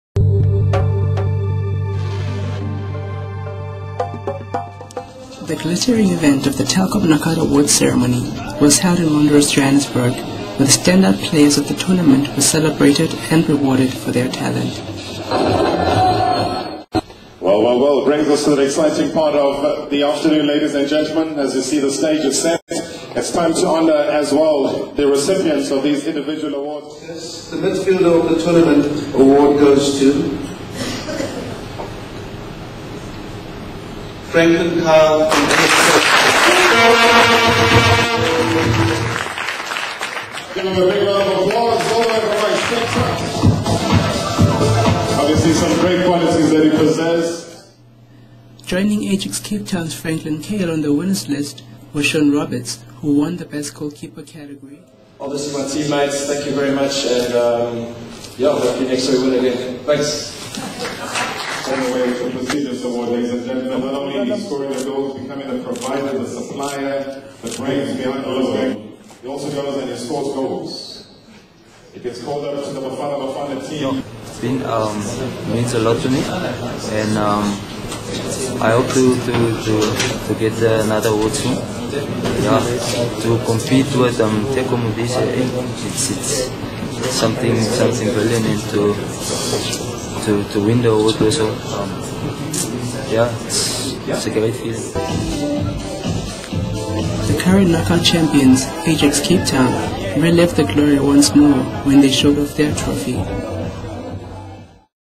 Watch the glitter and fun-fare at the Telkom Knockout Awards Ceremony.
The players who were rewarded for their individual brilliance in Ziwa Mo were Orlando Pirates Lucas Thwala and Ajax Cape Town stars Mabhuti Khenyeza, Franklin Cale and Sean Roberts.